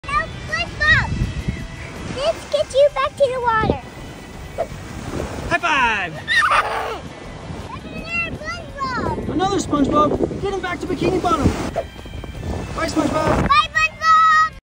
Baby Rescues Real Life SpongeBob! ❤️😭
1 Like = 1 Prayer for Spongebaby! Absolutely wholesome baby rescues real life SpongeBob.